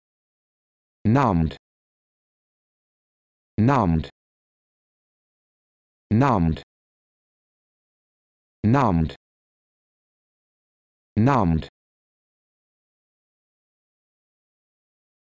How to Pronounce Nommed
This video shows you how to pronounce Nommed